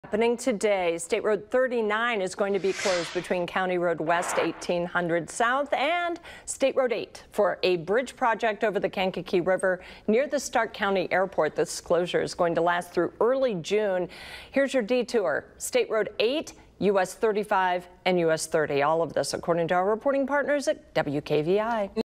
Portion of State Road 39 closed in LaPorte, Starke counties
State Road 39 is closing between County Road W. 1800 S. in LaPorte County and State Road 8 in Starke County for a bridge project over the Kankakee River.